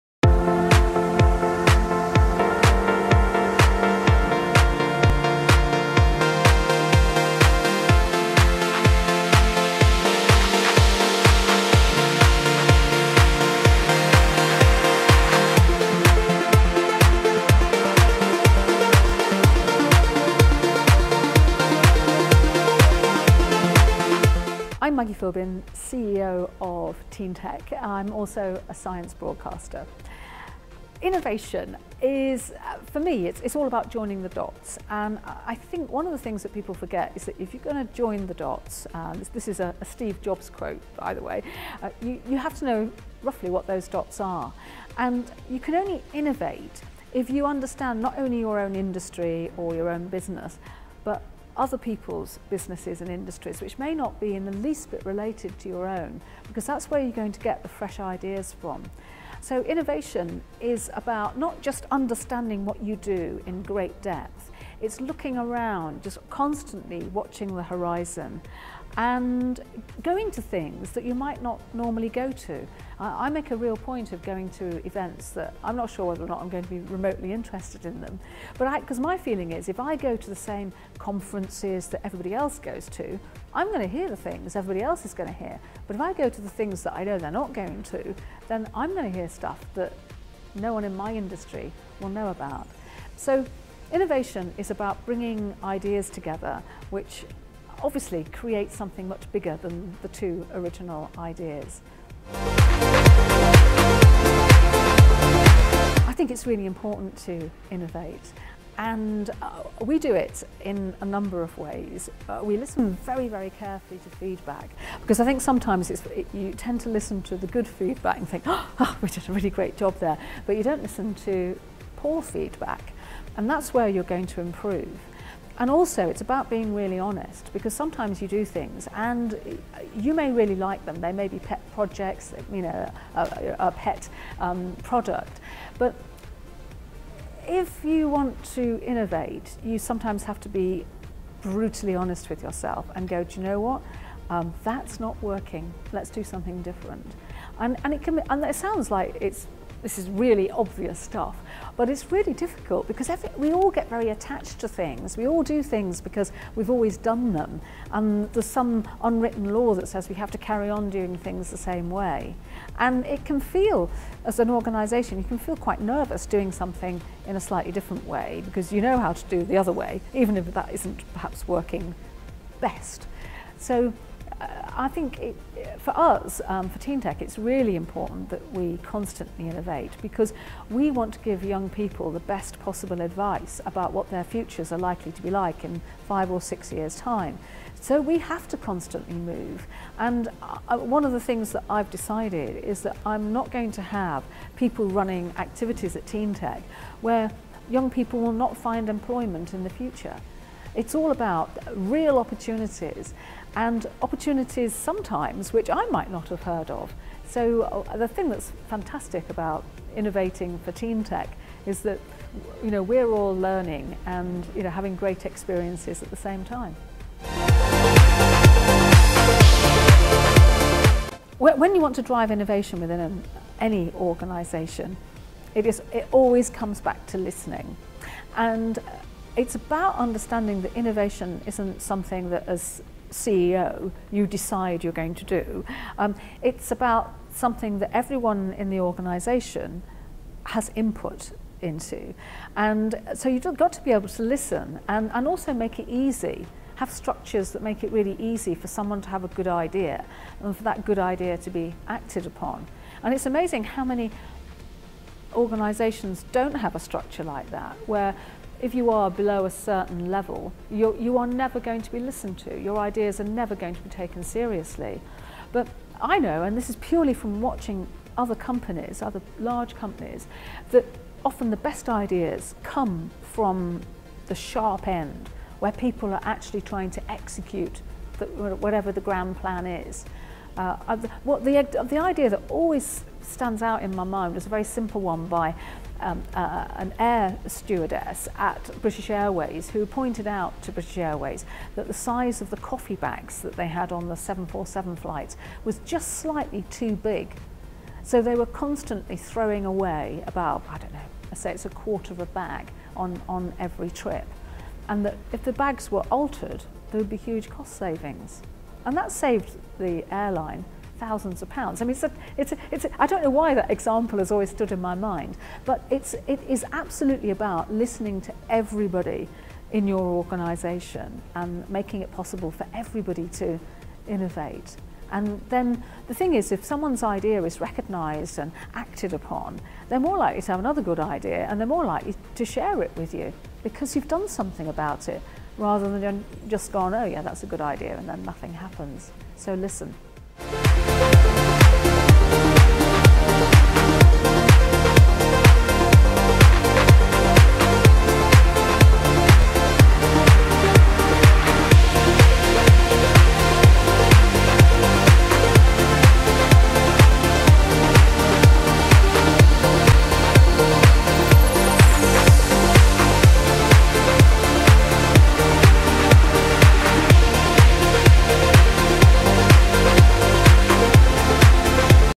Maggie Philbin discusses Innovation
Maggie Philbin, TV presenter and Chief Executive Officer of TeenTech discusses Innovation.

She answers three questions around innovation:
What Does Innovation Mean to You?
How could innovation continuously improve your business?

How can people in your organisation become more innovative, or continue to drive innovation?

Click on the video to hear what Maggie has to say.